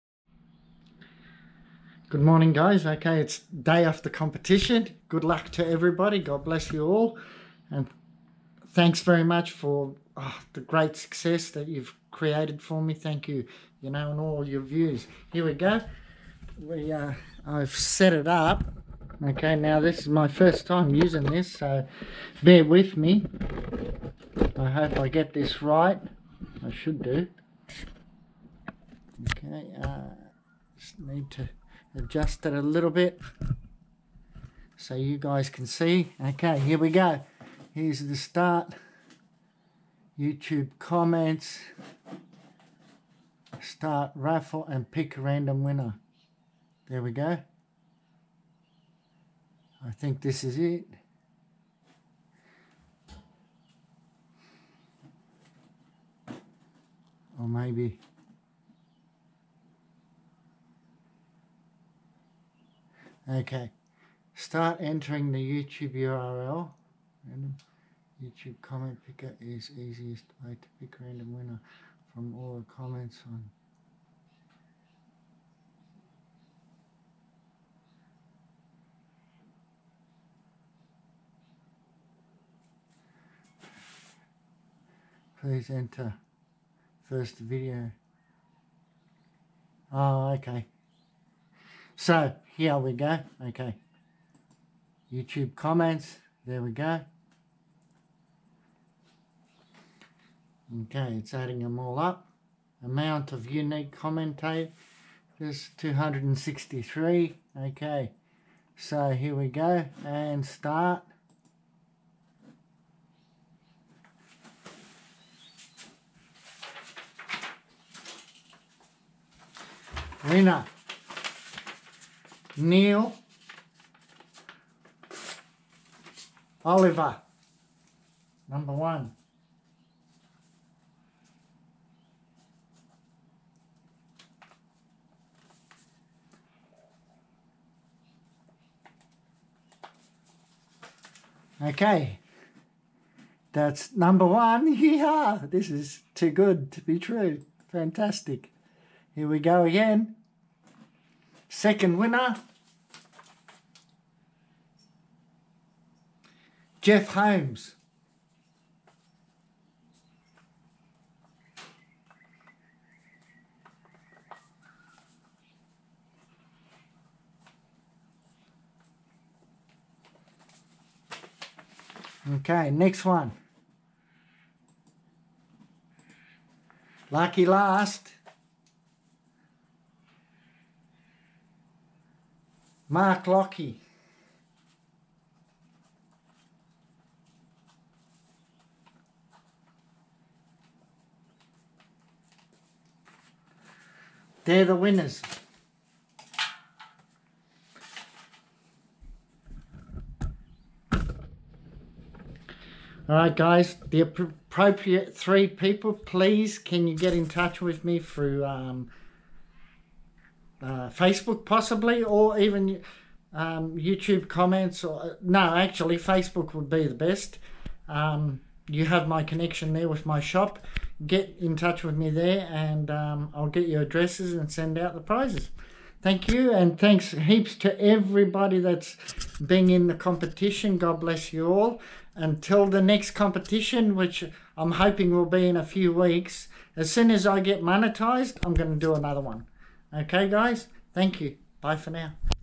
RESULTS OF GIVEAWAY Sapphire Mining Australia ,Tim's Adventures [ V: 149 ]
RESULTS OF GIVEAWAY VIDEO 146     ,Underground mining in Rubyvale, QLD, Australia